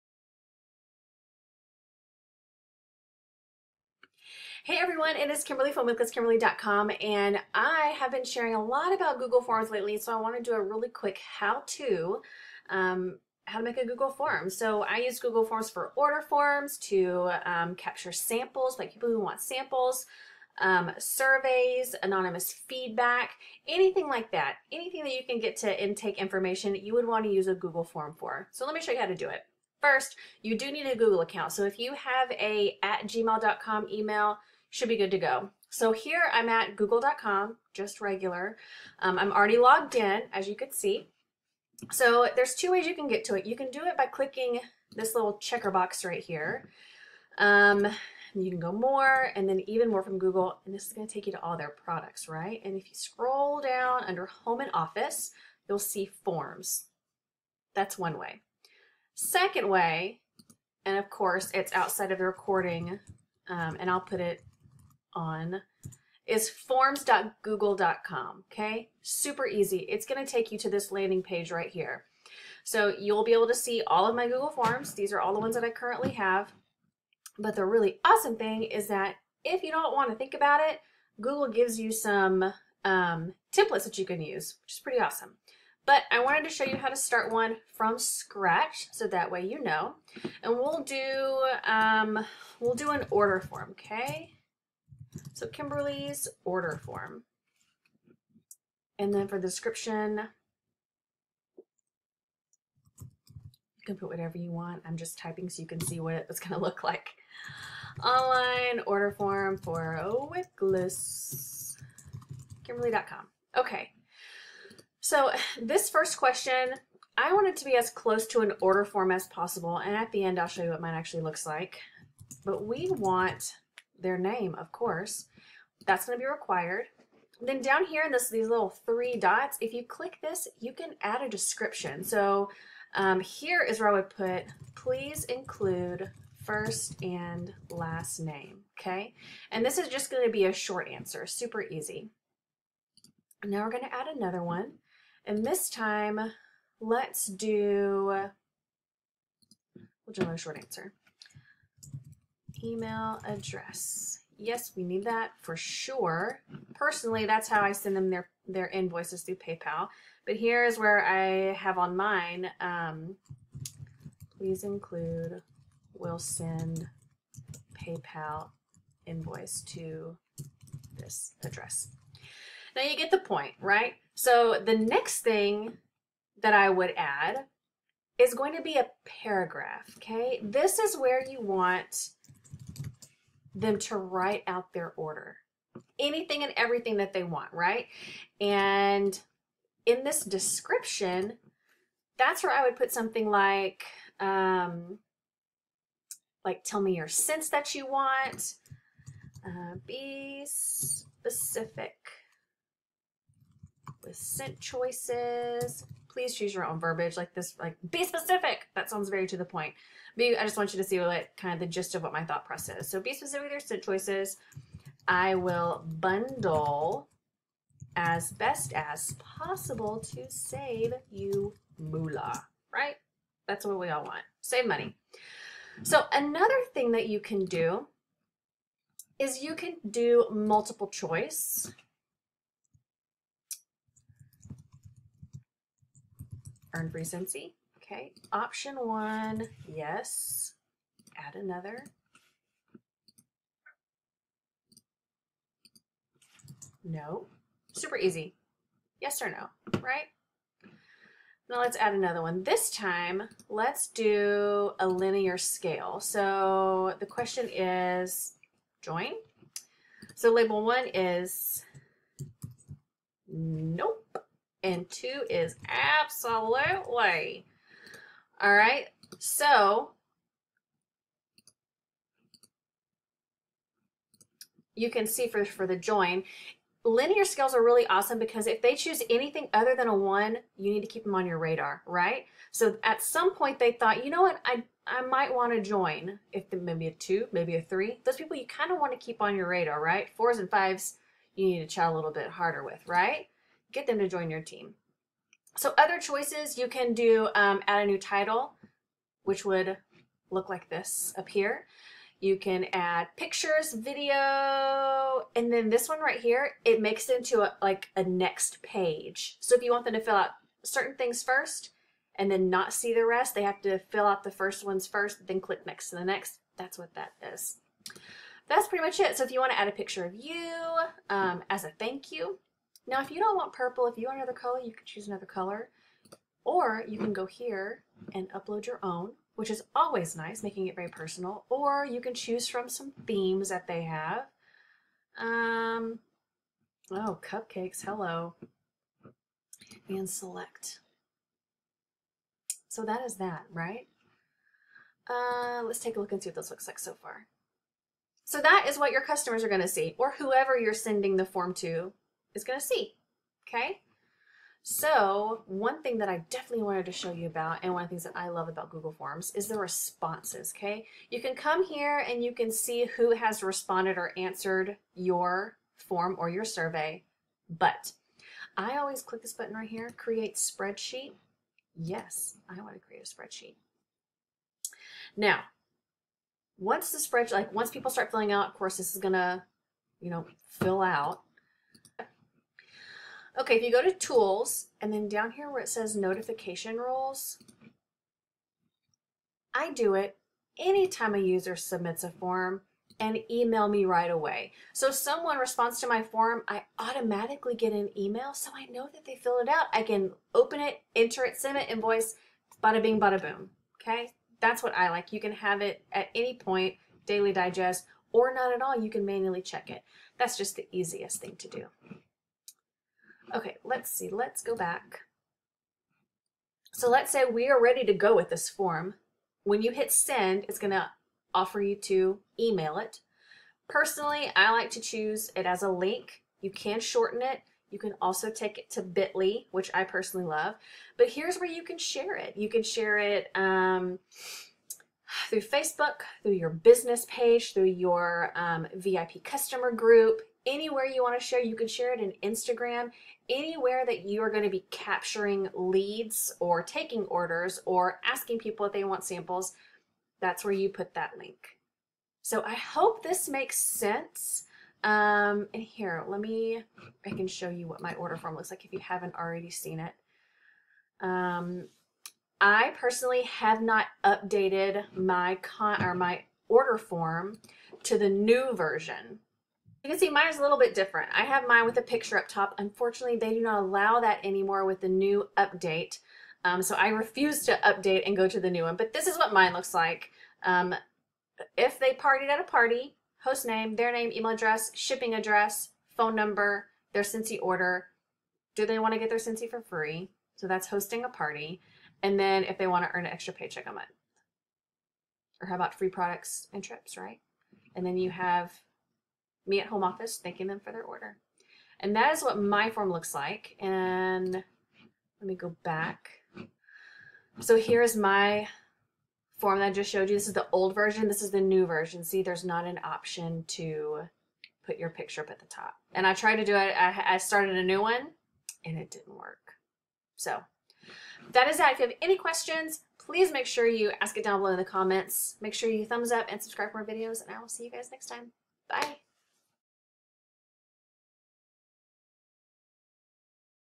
How To Make A Google Form
This video is a super quick tutorial on how to make a Google Form for your business. I personally use Google Forms for order forms, intake forms, sample requests, as a lead page, surveys, the list is endless.

I also mention that you cannot use the old type of Google Form which is incorrect. You can click on the question mark in the bottom right hand corner & choose to go back to the old forms.

Like it? Place an order!
Want it all?